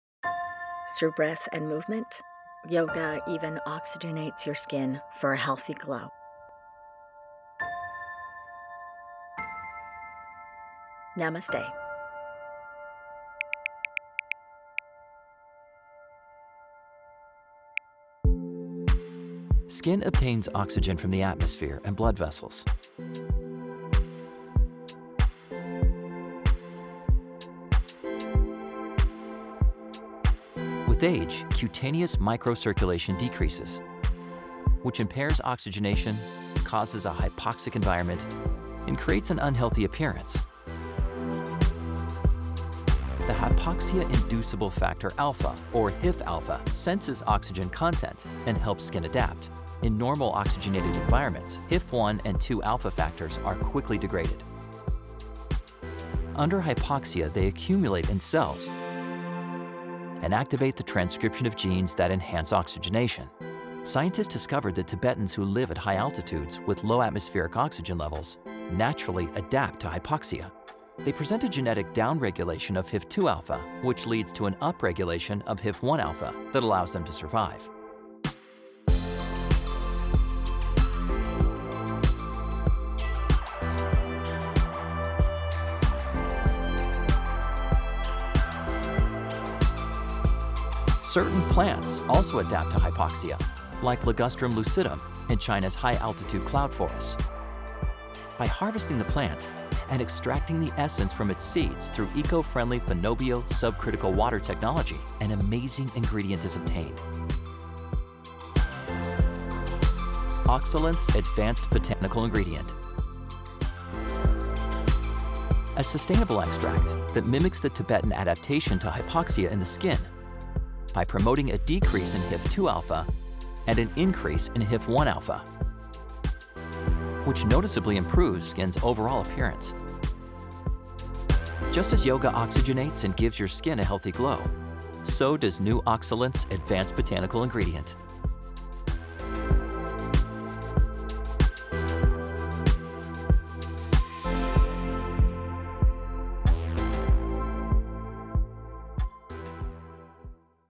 Oxylance™ advanced botanical ingredient
Botanical extract from Lingustrum lucidum plant that mimics the Tibetan genetic adaptation to hypoxia conditions for increased oxygen supply to the skin, resulting in glowing and healthier-looking “yoga skin”.